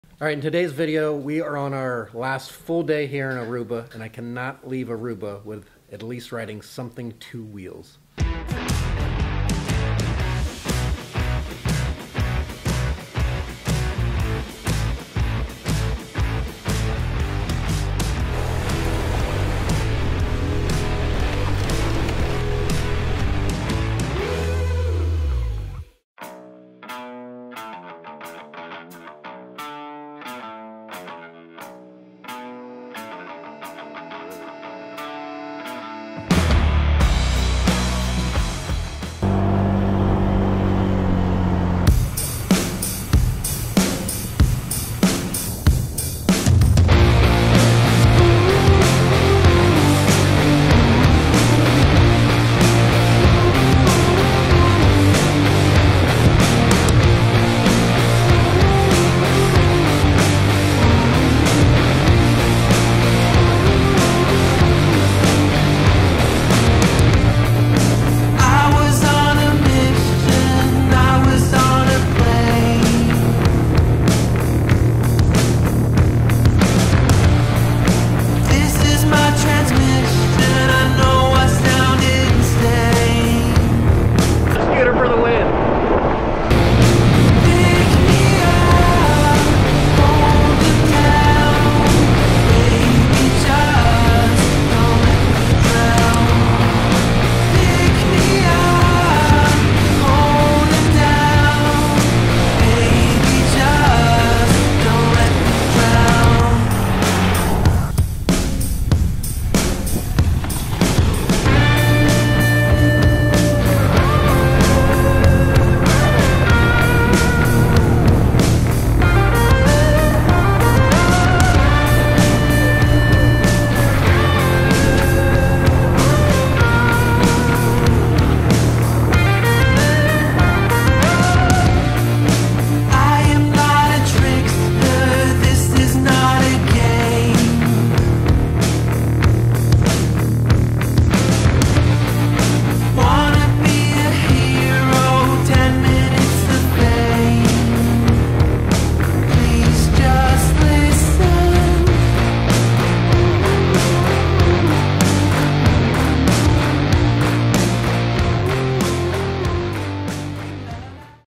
RENTING A SCOOTER IN ARUBA
For our 5 year anniversary we headed to Aruba. Of course I couldn't leave without riding 2 wheels.